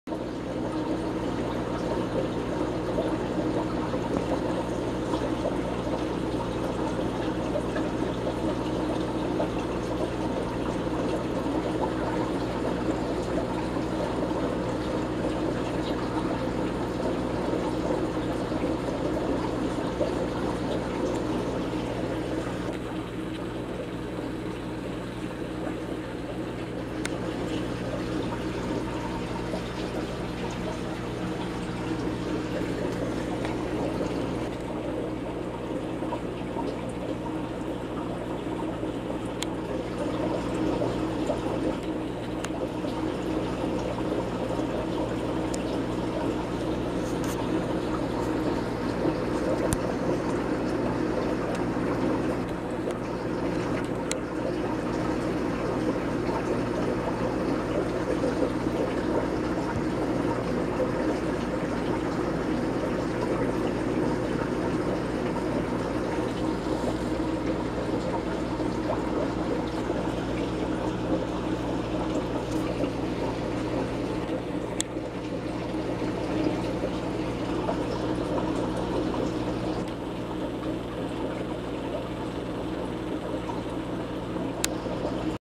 Polypterus Palmas Bichir
Got this little guy the other day, such a cool fish.